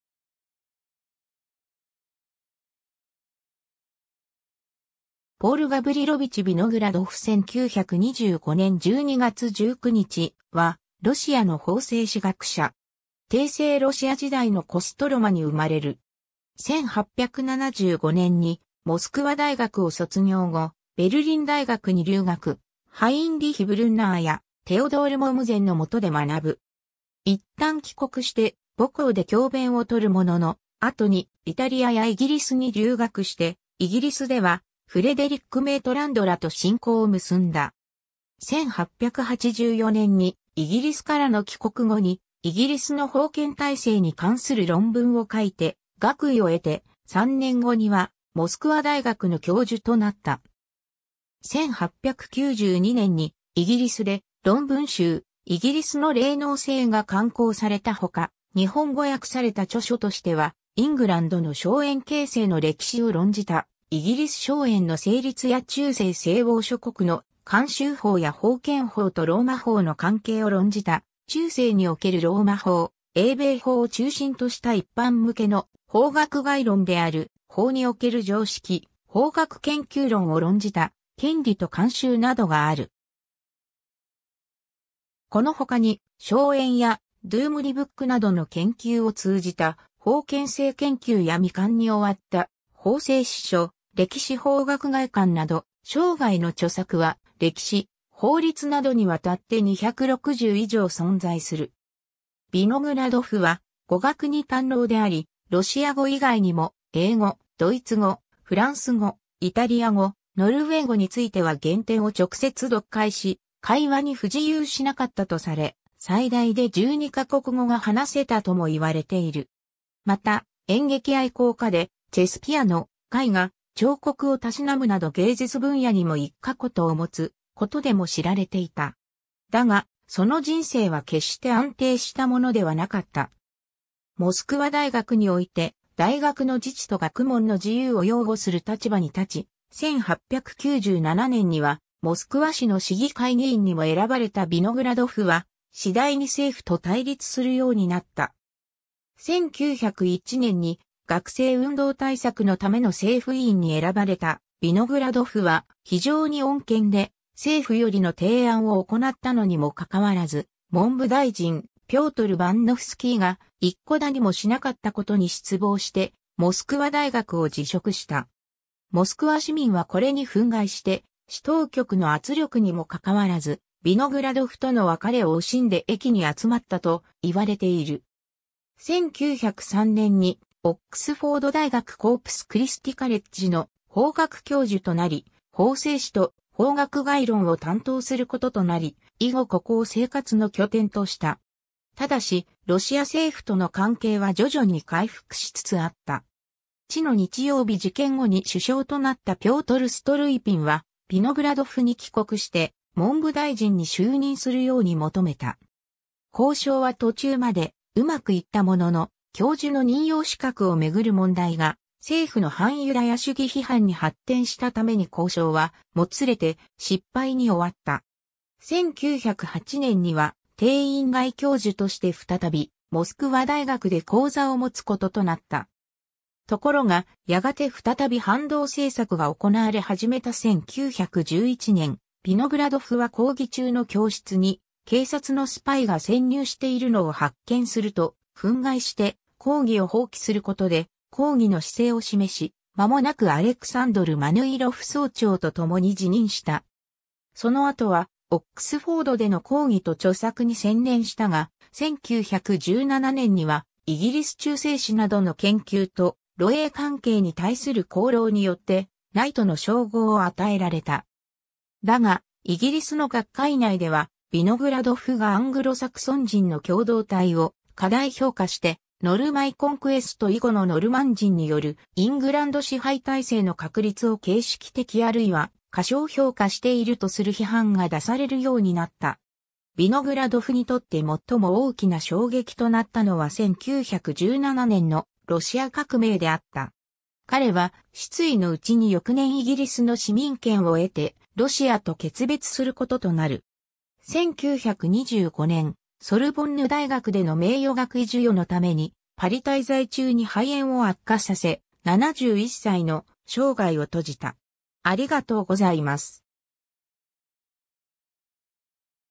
ポール・ヴィノグラドフ (法制史学者)
ポール・ガヴリロヴィチ・ヴィノグラドフ（パーヴェル・ガヴリロヴィチ・ヴィノグラドフ、露: Пáвел Гаври́лович Виногрáдов、英: Paul Gavrilovich Vinogradoff、1854年11月30日（ロシア暦11月18日） - 1925年12月19日）は、ロシアの法制史学者（後にイギリスに帰化）。
 帝政ロシア時代のコストロマに生まれる。
1875年にモスクワ大学を卒業後、ベルリン大学に留学、ハインリヒ・ブルンナーやテオドール・モムゼンの元で学ぶ。
一旦帰国して母校で教鞭を執るものの、後にイタリアやイギリスに留学して、イギリスではフレデリック・メイトランドらと親交を結んだ。
1884年にイギリスからの帰国後にイギリスの封建体制に関する論文を書いて学位を得て、3年後にはモスクワ大学の教授となった。
 1892年にイギリスで論文集『イギリスの隷農制』（Villainage in England）が刊行されたほか、日本語訳された著書としてはイングランドの荘園形成の歴史を論じた『イギリス荘園の成立』（1904年）や中世西欧諸国の慣習法や封建法とローマ法の関係を論じた『中世におけるローマ法』（1909年）、英米法を中心とした一般向けの法学概論である『法における常識』（1914年）、法学研究論を論じた『権利と慣習』（1925年）などがある。
この他に荘園やドゥームディブックなどの研究を通じた封建制研究や未完に終わった法制史書『歴史法学概観』（1920年以後）など、生涯の著作は歴史・法律などにわたって260以上存在する。
 ヴィノグラドフは語学に堪能でありロシア語以外にも英語・ドイツ語・フランス語・イタリア語・ノルウェー語については原典を直接読解し会話に不自由しなかったとされ、最大で12ヶ国語が話せたとも言われている。
 また、演劇愛好家でチェス・ピアノ・絵画・彫刻を嗜むなど芸術分野にも一家言を持つことでも知られていた。
 だが、その人生は決して安定したものではなかった。
モスクワ大学において大学の自治と学問の自由を擁護する立場に立ち、1897年にはモスクワ市の市議会議員にも選ばれたヴィノグラドフは次第に政府と対立するようになった。
1901年に学生運動対策のための政府委員に選ばれたヴィノグラドフは、非常に穏健で政府よりの提案（教授同席の元での非政治的な会合以外の学生の会合を禁じる）を行ったのにも拘らず、文部大臣ピョートル・ヴァンノフスキーが一顧だにもしなかったことに失望してモスクワ大学を辞職した。
モスクワ市民はこれに憤慨して市当局の圧力にも拘らず、ヴィノグラドフとの別れを惜しんで駅に集まったと言われている。
1903年にオックスフォード大学コープス・クリスティ・カレッジの法学教授となり、法制史と法学概論を担当することとなり、以後ここを生活の拠点とした。
 ただし、ロシア政府との関係は徐々に回復しつつあった。
血の日曜日事件後に首相となったピョートル・ストルイピンは、ヴィノグラドフに帰国して文部大臣に就任するように求めた。
交渉は途中まで上手くいったものの、教授の任用資格を巡る問題が政府の反ユダヤ主義批判に発展したために交渉はもつれて失敗に終わった。
 1908年には定員外教授として再びモスクワ大学で講座を持つこととなった。
ところが、やがて再び反動政策が行われ始めた1911年、ヴィノグラドフは講義中の教室に警察のスパイが潜入しているのを発見すると憤慨して講義を放棄することで抗議の姿勢を示し、間もなくアレクサンドル・マヌイロフ総長とともに辞任した。
その後はオックスフォードでの講義と著作に専念したが、1917年にはイギリス中世史などの研究と露英関係に対する功労によってナイトの称号を与えられた。
だが、イギリスの学会内ではヴィノグラドフがアングロ・サクソン人の共同体を過大評価してノルマン・コンクエスト以後のノルマン人によるイングランド支配体制の確立を形式的あるいは過小評価しているとする批判が出されるようになった。
 ヴィノグラドフにとって最も大きな衝撃となったのは1917年のロシア革命であった。
彼は失意のうちに翌年イギリスの市民権を得てロシアと訣別することとなる。
1925年、ソル...